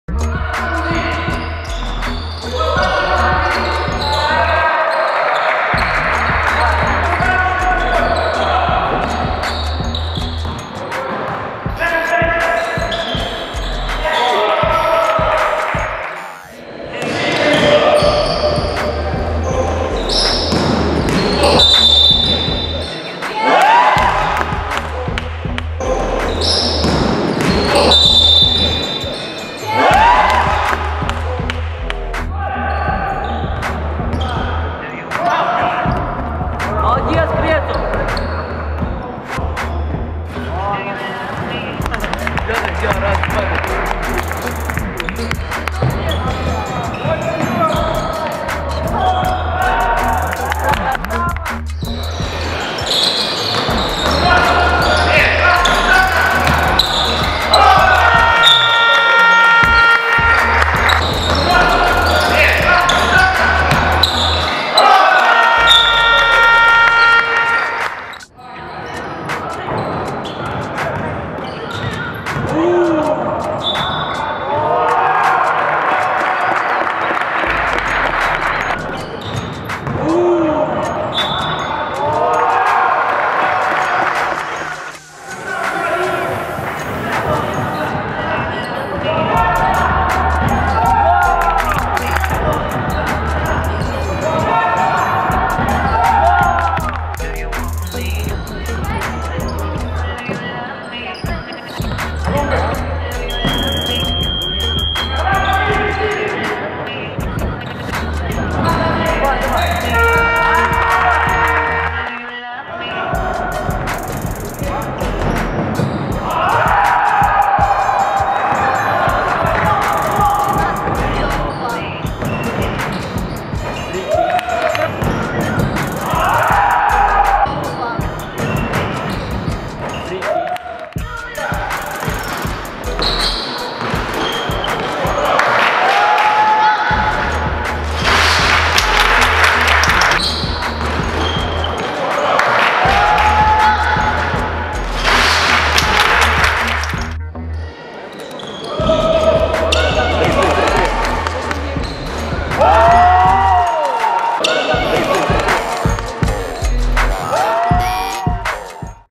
November & December: Trojans-Speranța Top 10 Plays [HD]
Trojans-Speranța Top Plays from November & December
National Championship
National Division, Season 2015/2016
First stage, first round